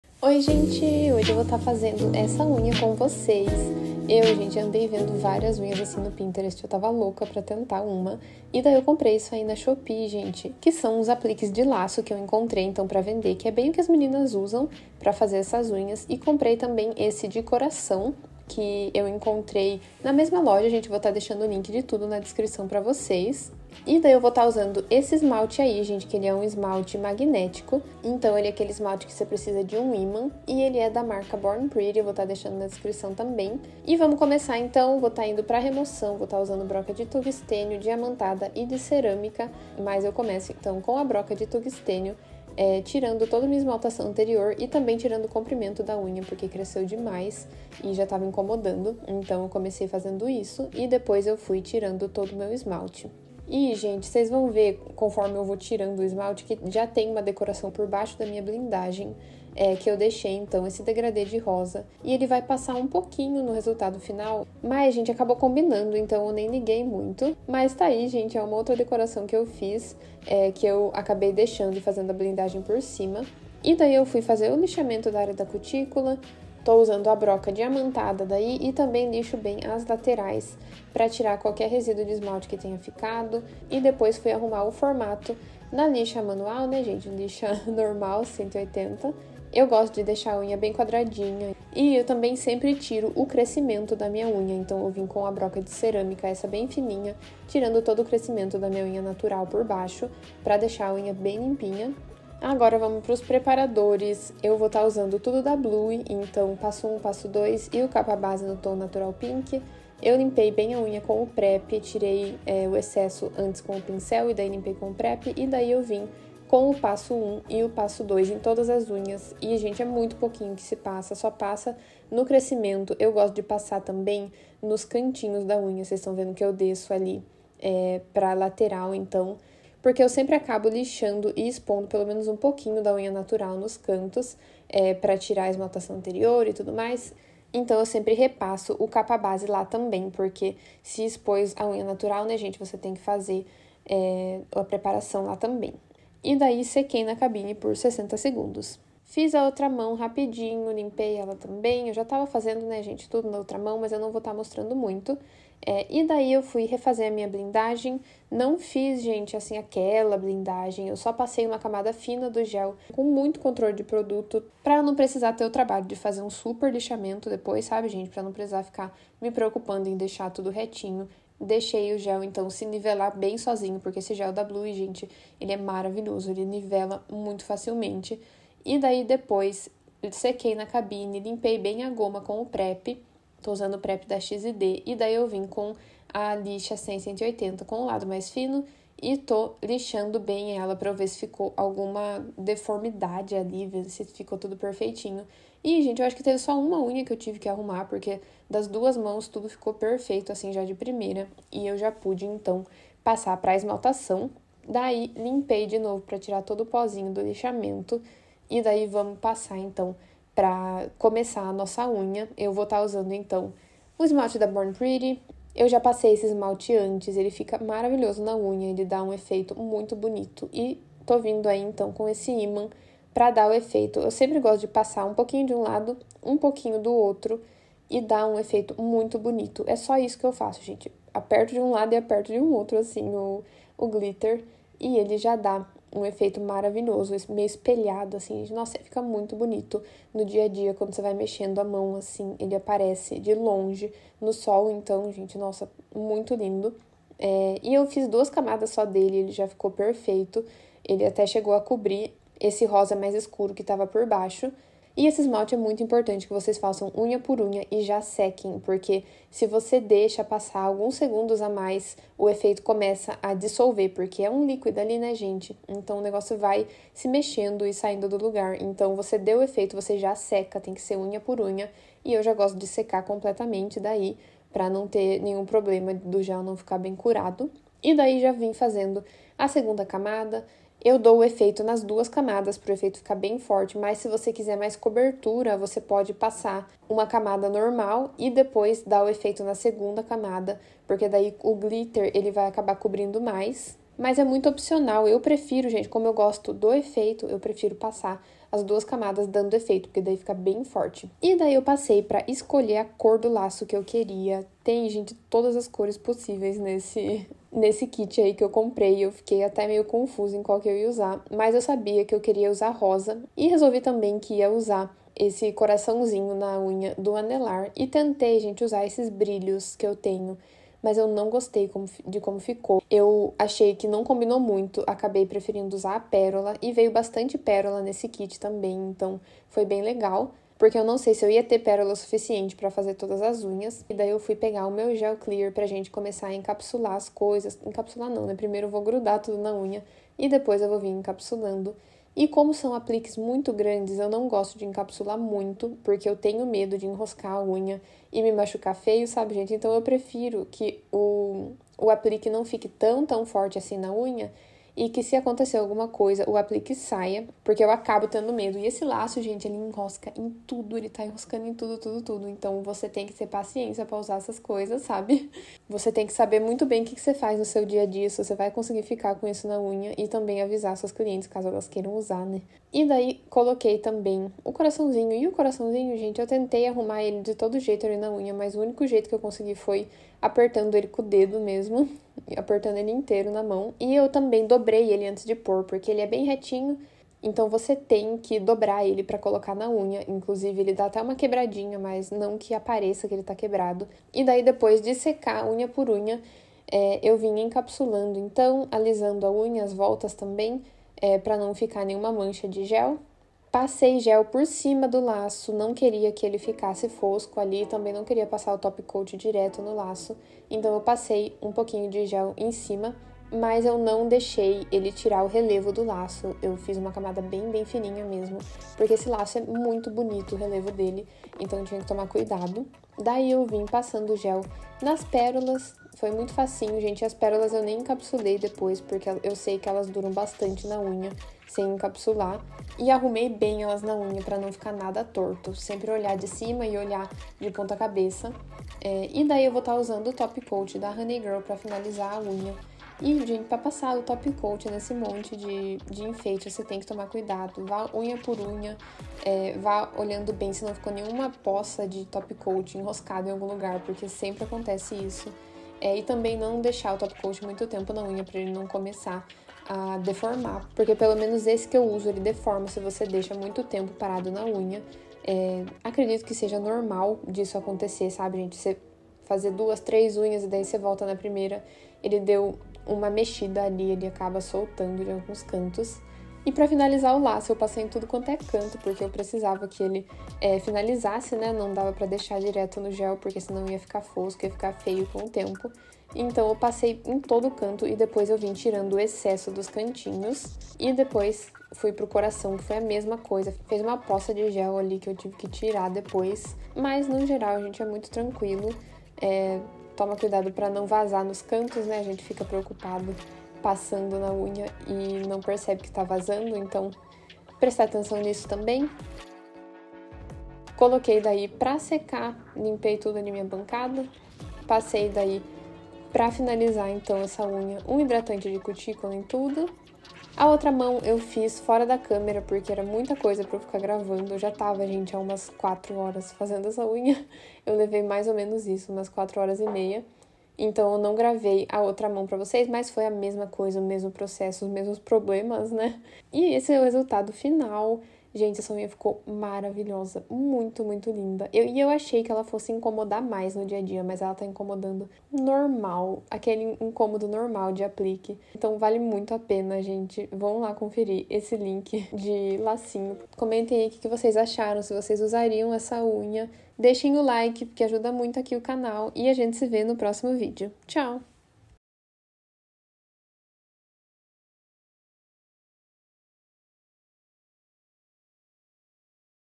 Unha em gel rosa com laço e pérolas 💅| Estilo coquette 🎀✨
CURSO COMPLETO NAIL DESIGNER INICIANTE POR 60,00 reais
ABRA SEU SALÃO EM CASA!

Oii gente! Hoje vim mostrar pra vocês mais uma unha aqui no salão, espero que gostem!